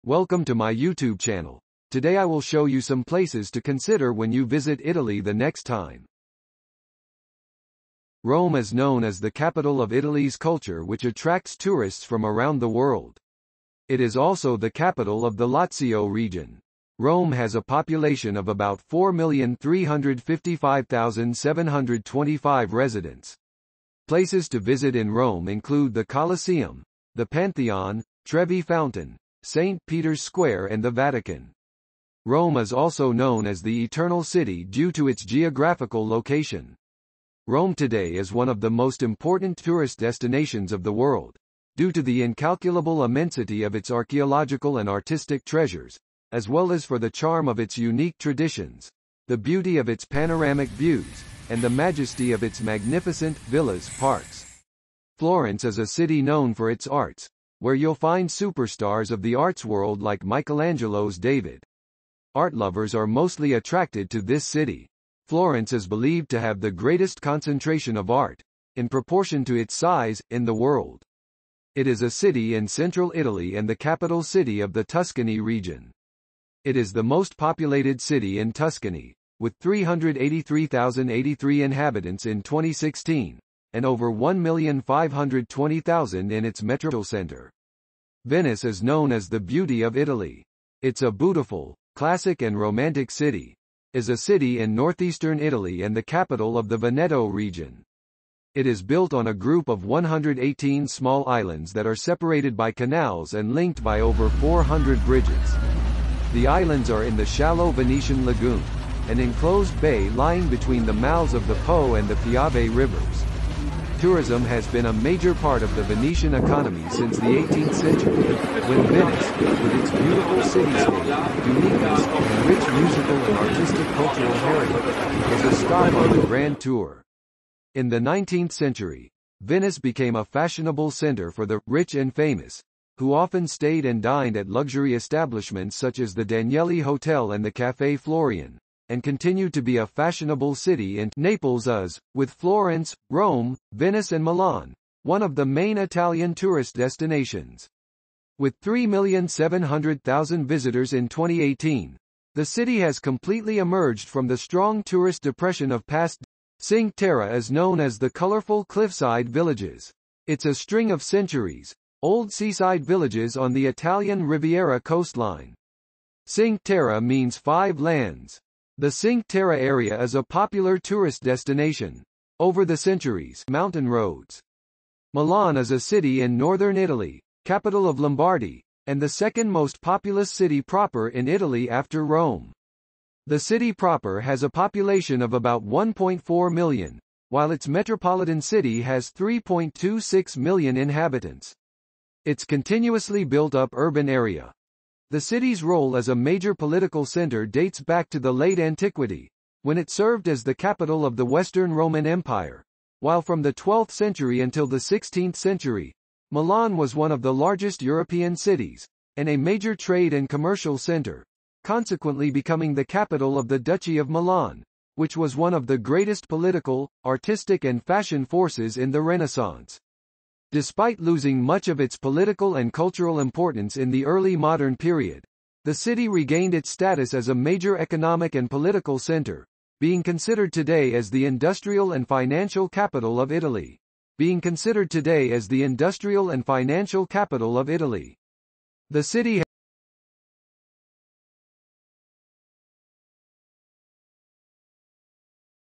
Places to visit in Italy 2023
This video gives you an idea of some wonderful places to consider when visiting Italy the next trip.